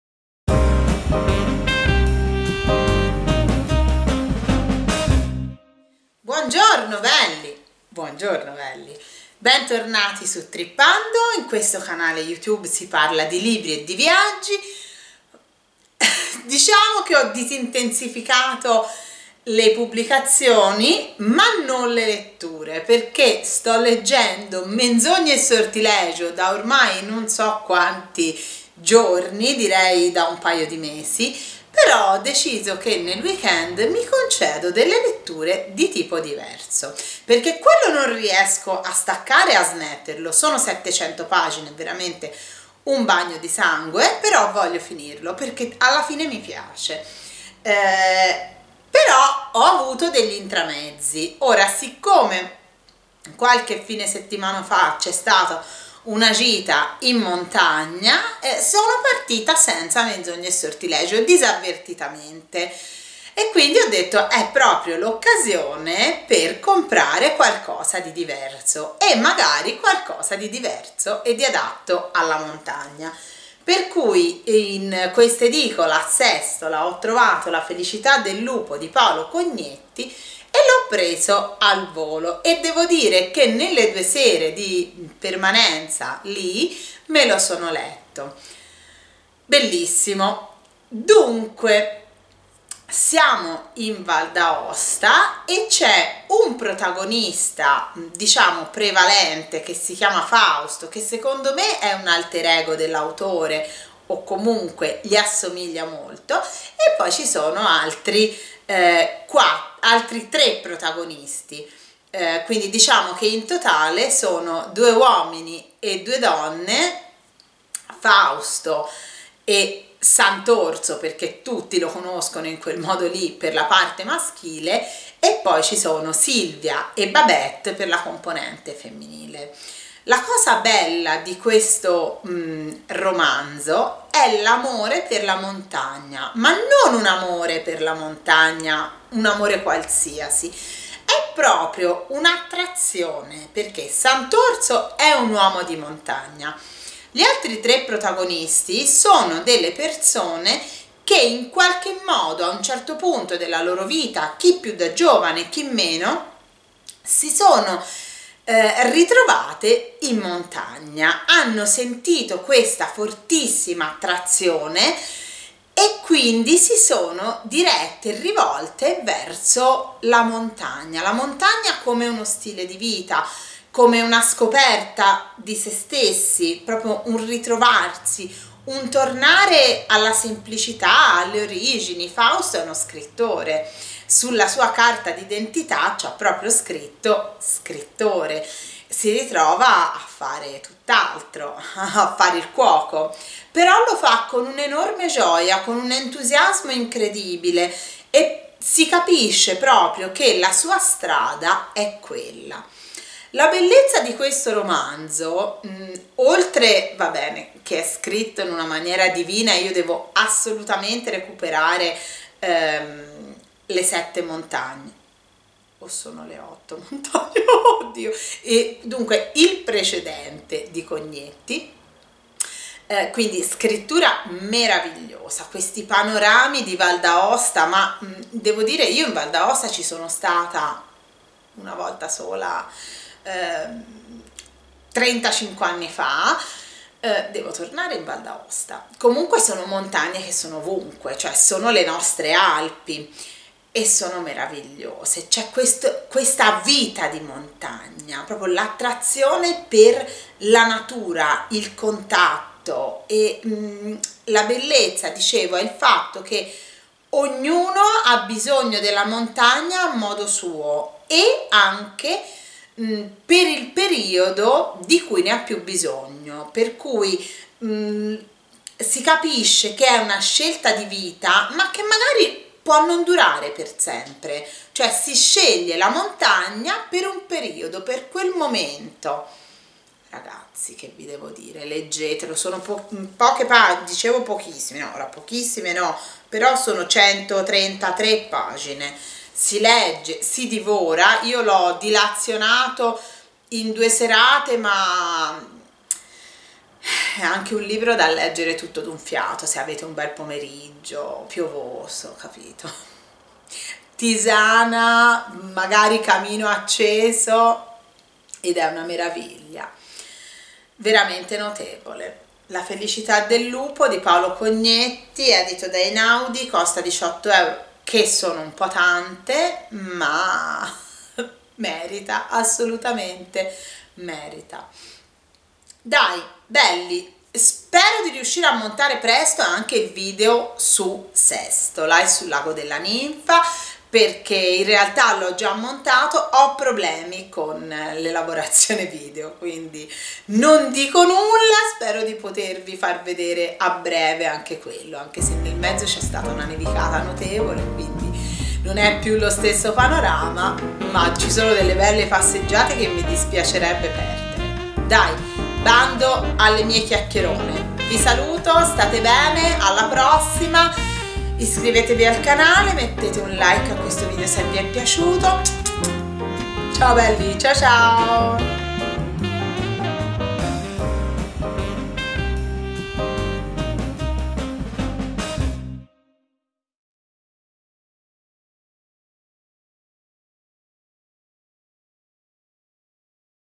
La felicità del lupo - Paolo Cognetti - Montagna
In questo video ti racconto il romanzo La felicità del lupo, di Paolo Cognetti. Attraverso il protagonista e gli altri personaggi vediamo il vivere in montagna, quello che la montagna può apportare come valore aggiunto nella vita di chi è alla ricerca di sé stesso o di un cambiamento.
Per acquistare il libro